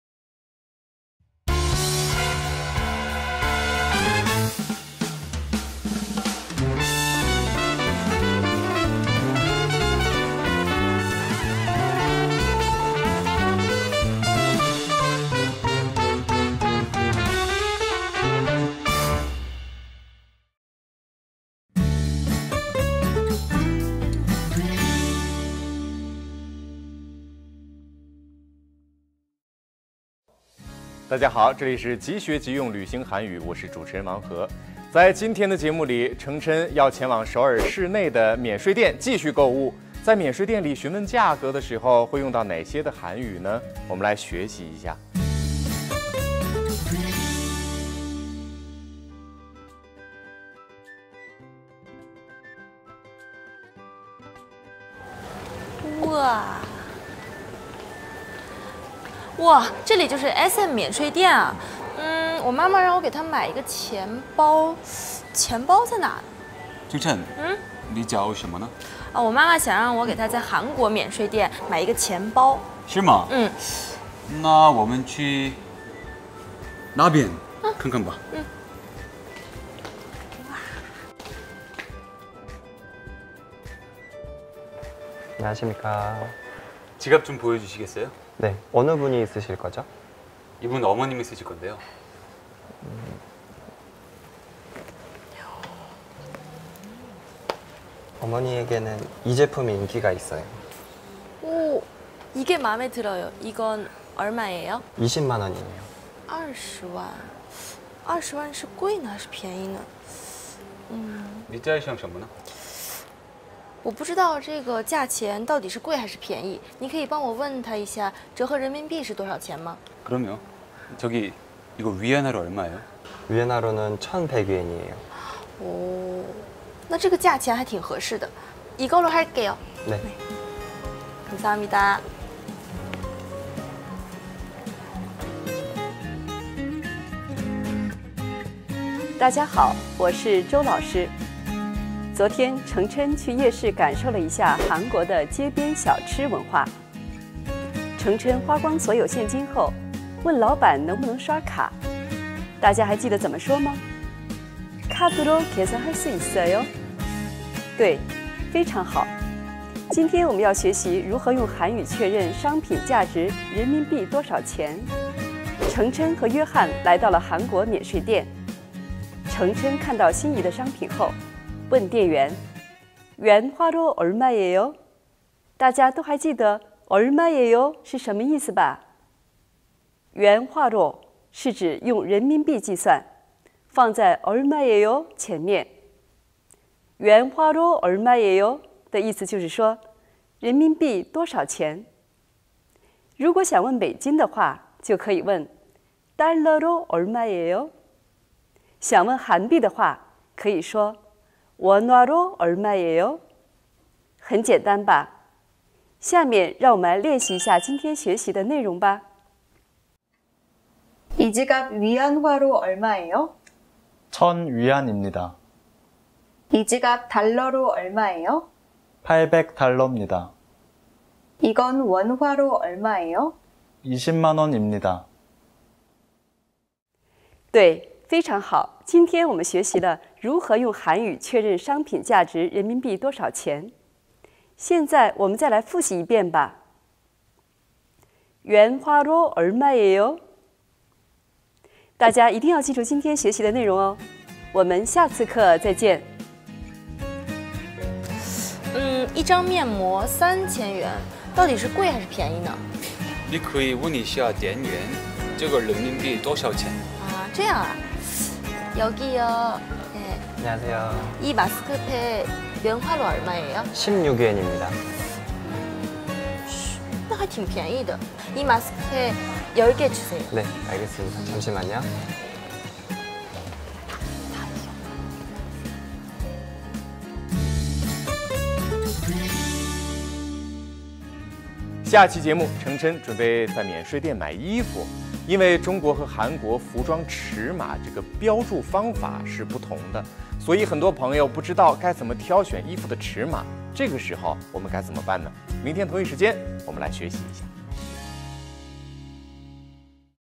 바로 배워 바로 쓰는 여행 한국어 23편/ 即学即用旅行韩语 第二十三编
실생활에서 바로 쓰이는 한국 여행에 꼭 필요한 한국어 강의!

故事性强，寓教于乐，中间穿插韩语老师的生动讲解和最标准的母语发音示范，保证观众学到最原汁原味的生活韩语!